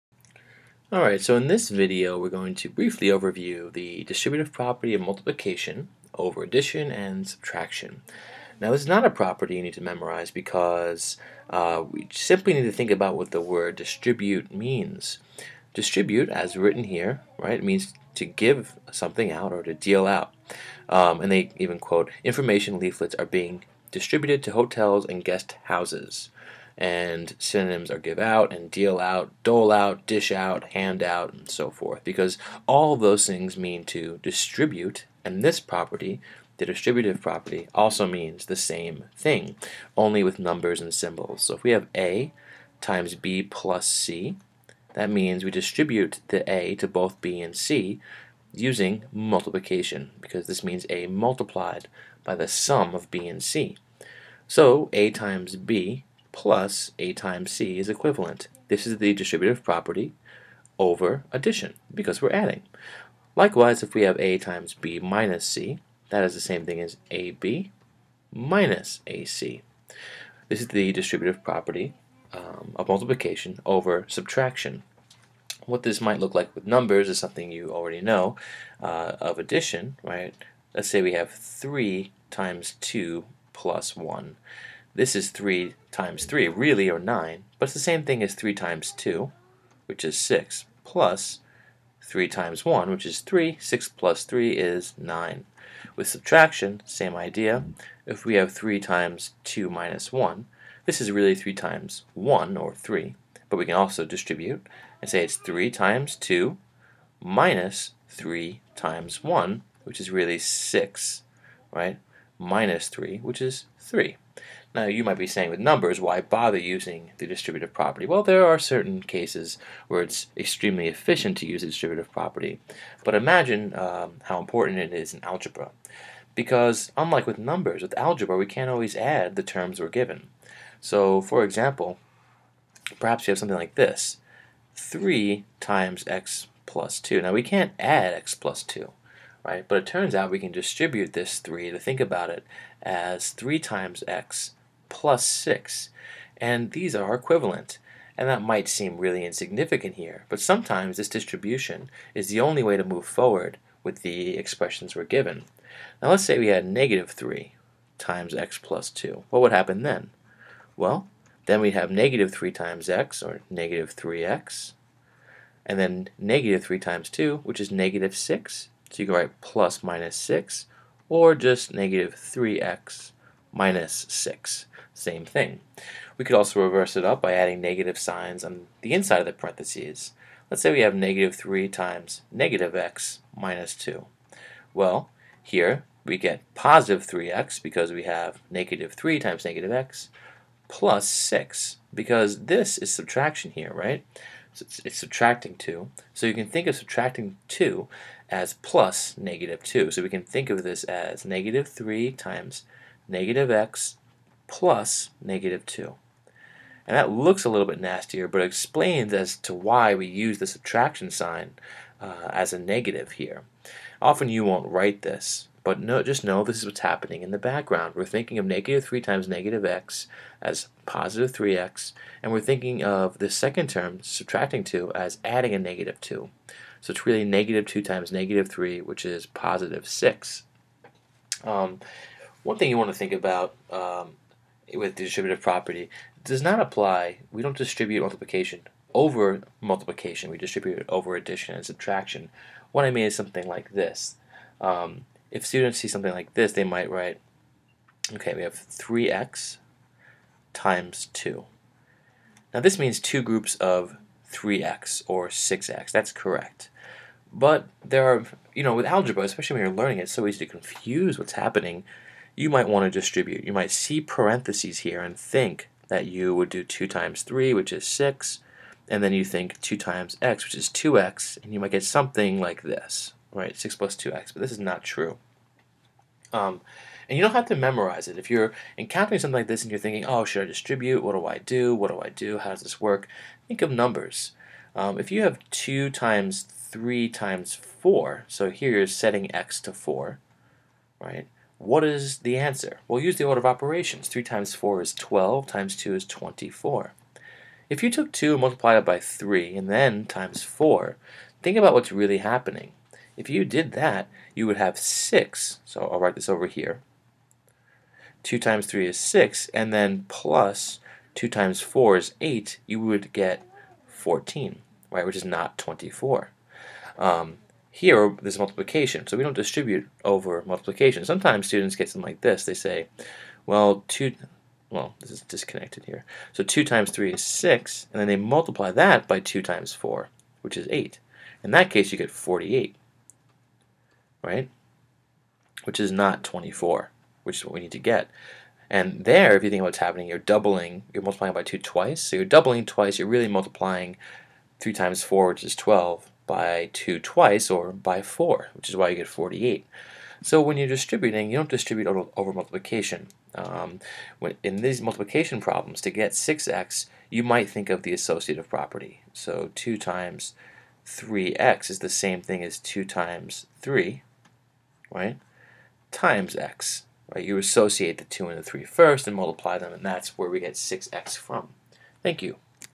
Distributive Property Overview
In this video we review different aspects of the distributive property of multiplication over addition and subtraction